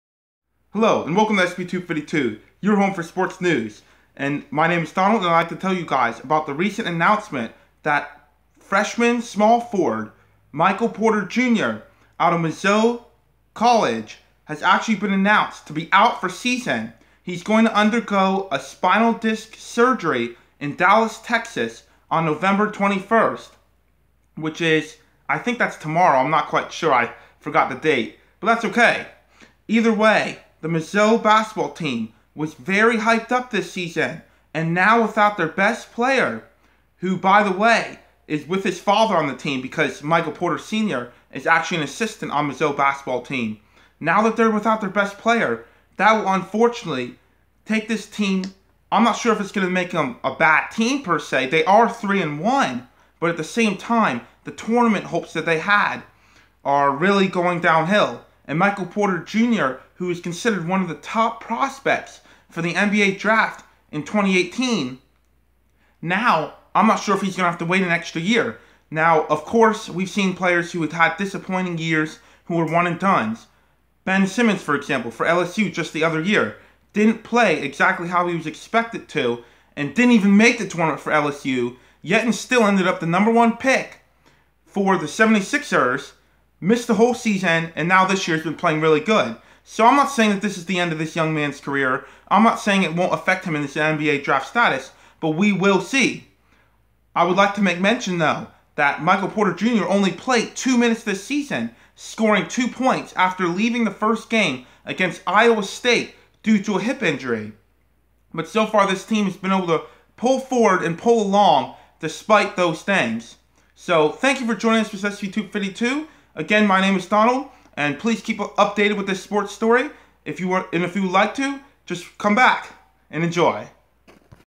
Michael Porter Jr. Back Surgery
Today, it was announced by Mizzou's official Twitter page that theri star player and top recruit, Michael Porter Jr. would miss the rest of the season. He is scheduled to have a disc surgery that will shelve him for the months to come.